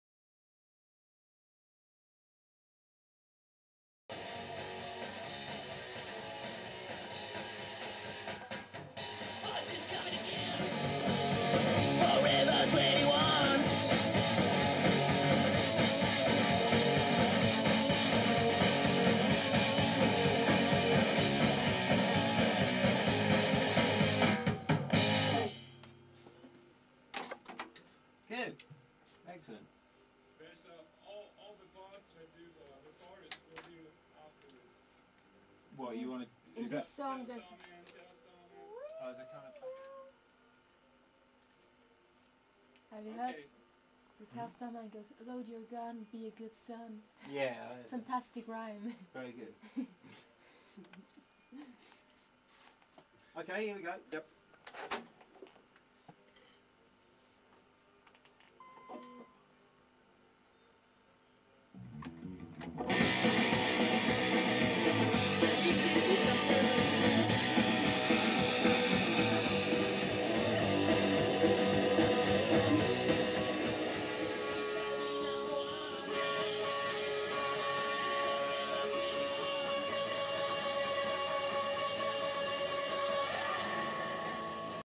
the SPLASH FOUR at TOE RAG STUDIOS 1996
A recording session with Liam Watson at Toe Rag Studios, London August 1996.
'Fuckin' Brilliant !'